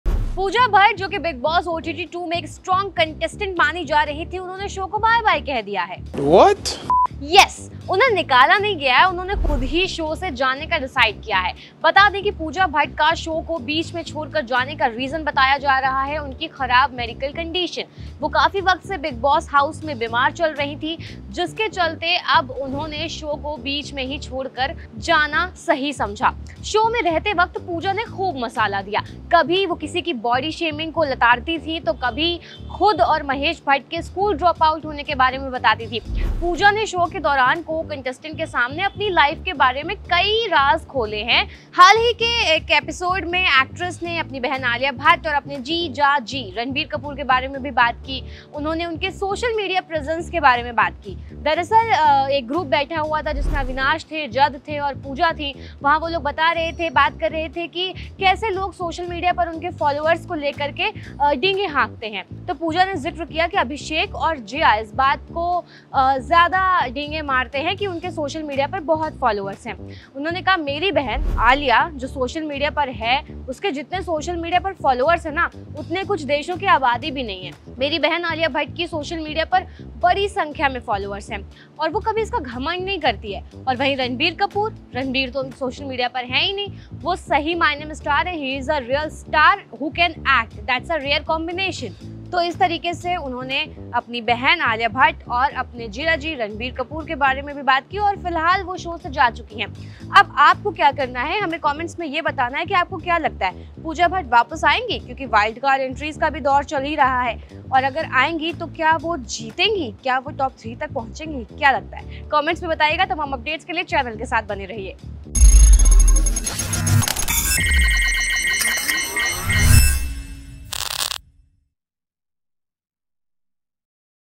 Pooja Bhatt ने Bigg Boss में की Alia और Ranbir की बात कर क्यों छोड़ दिया Show ? | ENT LIVE
शो में रहते हुए Pooja Bhatt ने खूब मसाला दिया, तो अब शो को बीच में क्यों छोड़कर गई ?

Producer - Bhavna
Cameraperson -
Editor- Honey